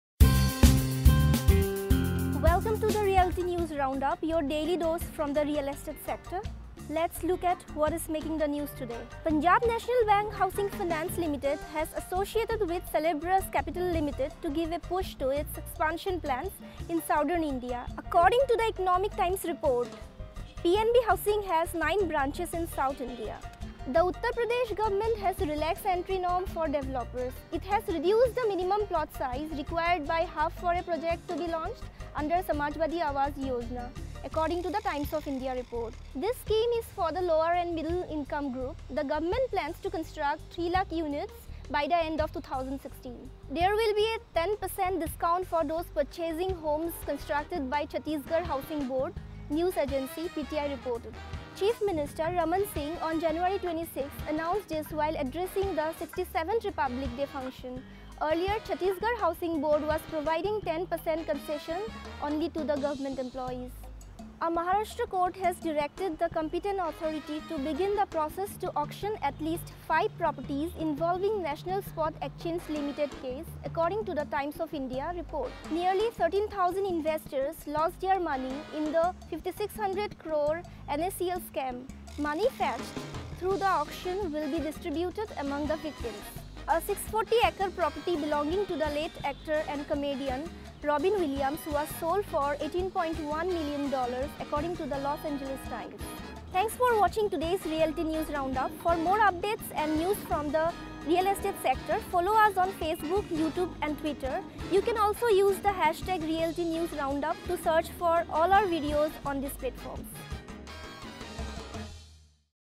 Realty News Round Up 27th January, 2016
RealtyNewsRounup Punjab National Bank (PNB) Housing Finance Limited has associated with Celebrus Capital Limited to give a push to its expansion plans in Southern India, according to The Economic Times report. PNB Housing has nine branches in South India.
Watch this video for more...